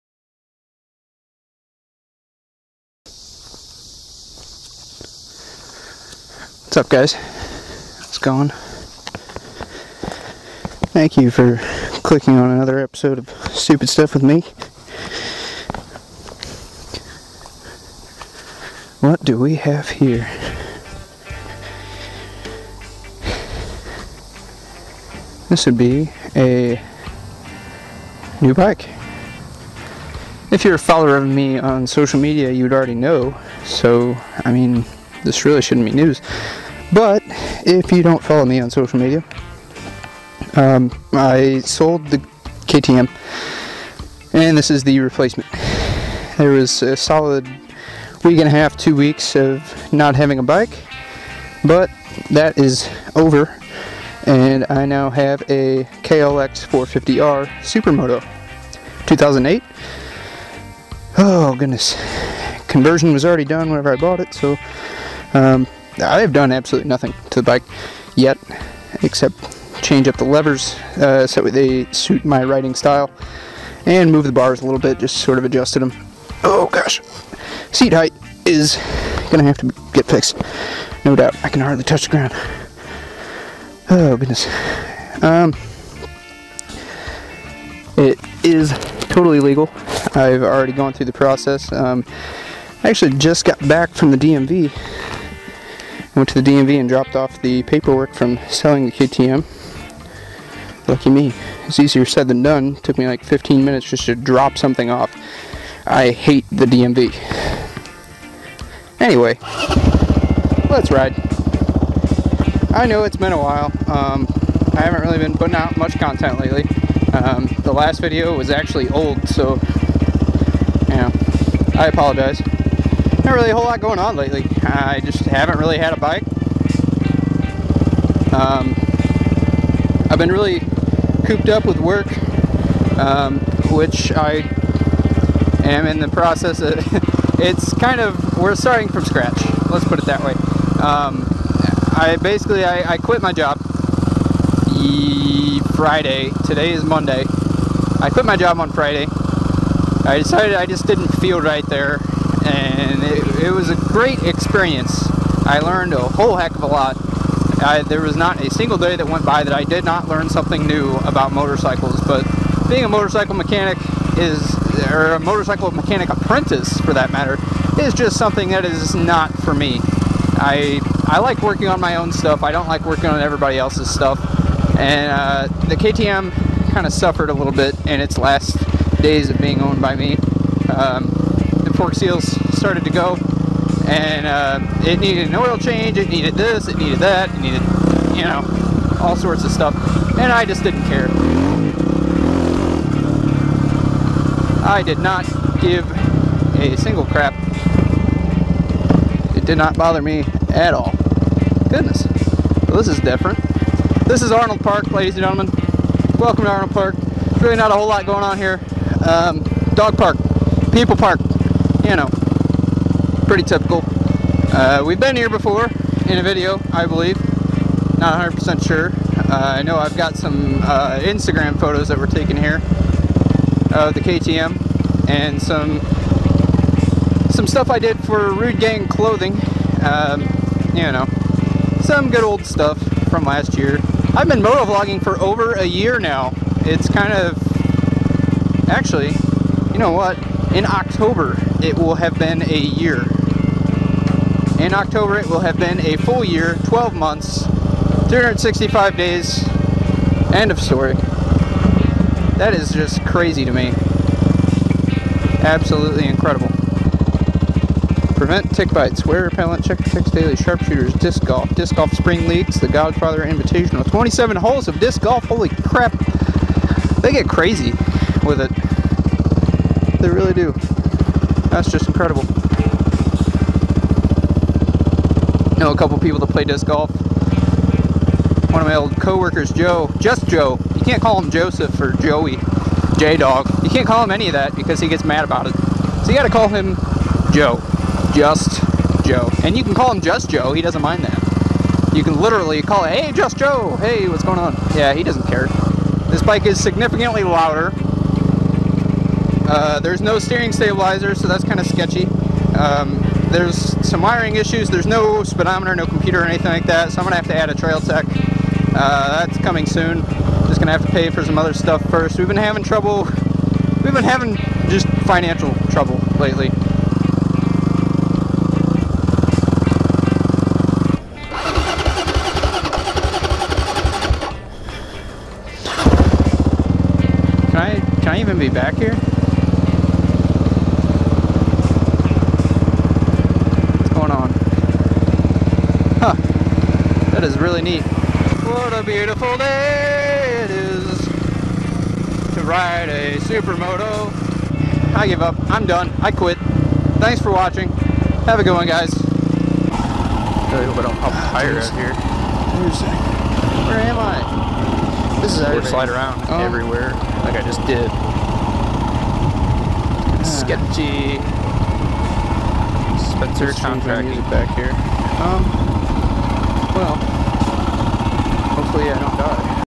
KLX450R Supermoto // Vlog 030
I sold my beloved KTM and bought a green bike..